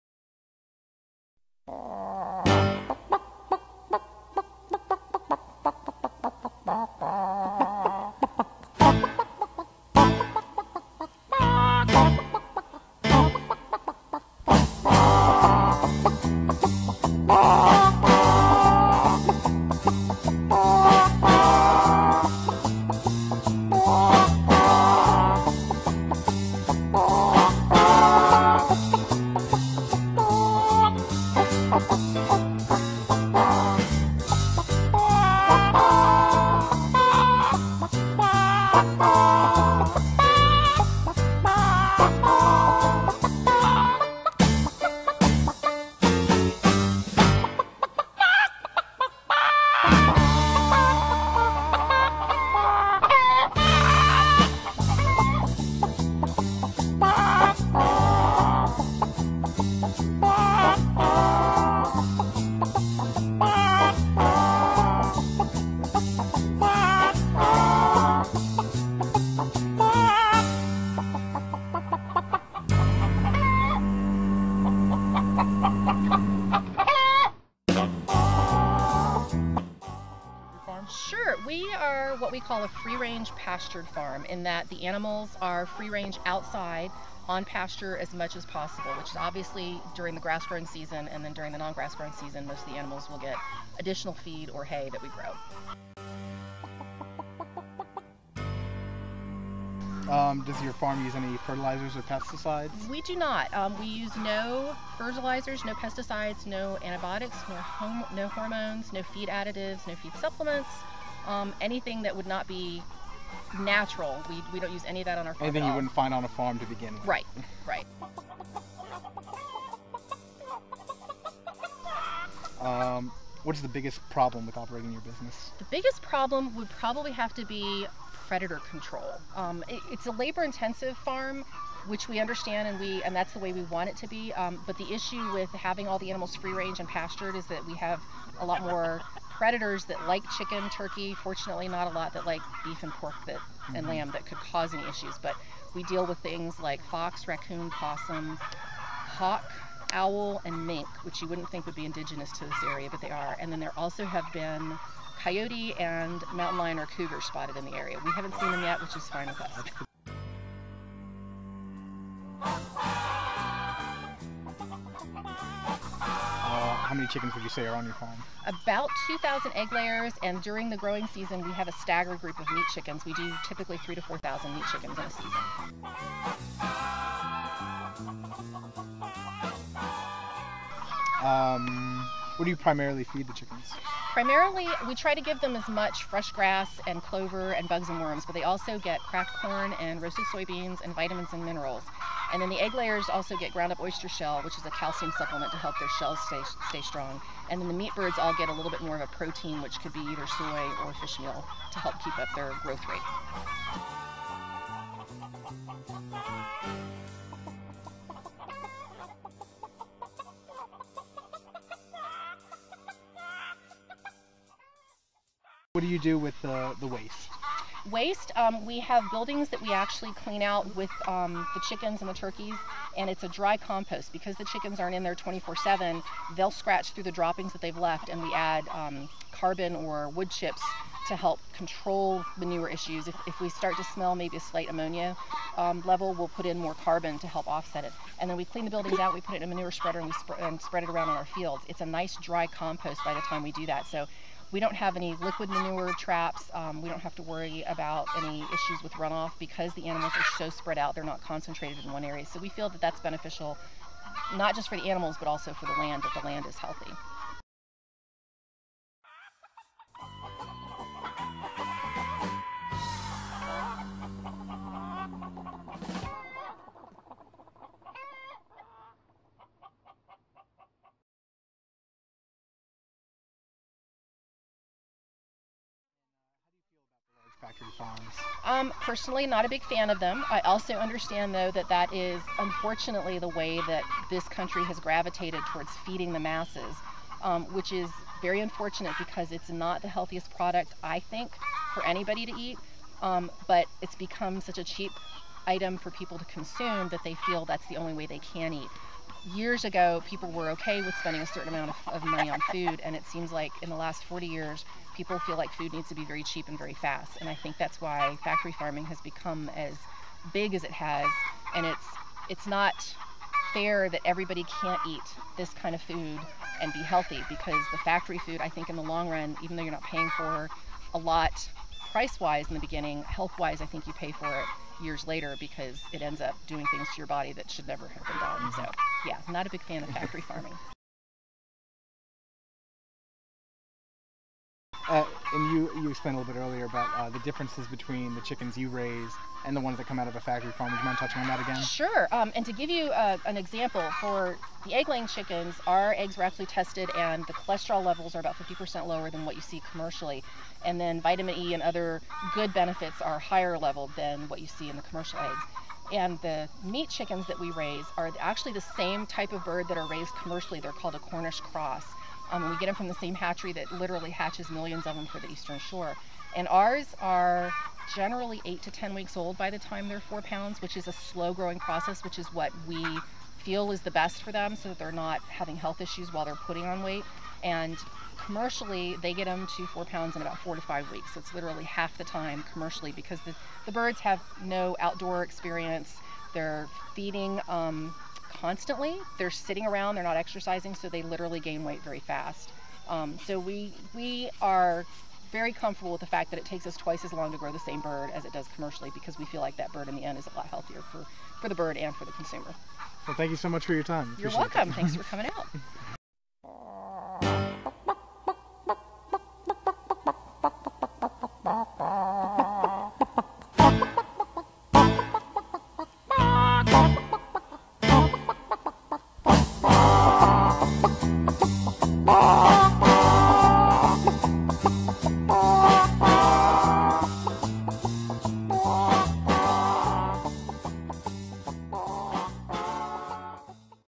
Chicken Movie Econ376
An awesome movie covering the costs and benefits associated with chicken farming... This movie should get an A .